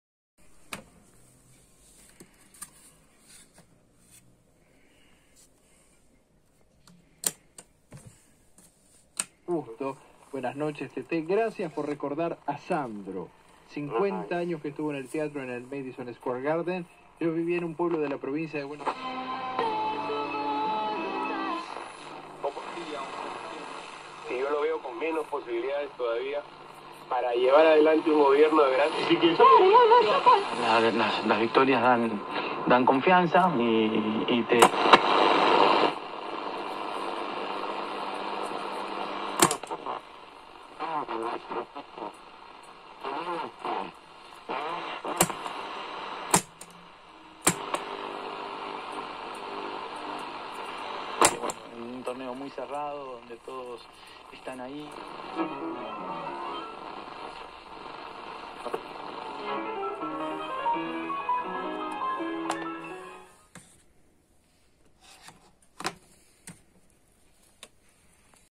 Radio Tonomac Súper Platino
Radio Tonomac Super Platino 6 bandas
Con Mapamundi
Funciona en todas sus bandas
La antena está completa , sube y baja bien
Le faltan unas mini perillitas que se aprecian en las fotos Pero no impiden esto su buen funcionamiento
Trae su cable a 220 para probarla al retirar.
Miren el siguiente video que hicimos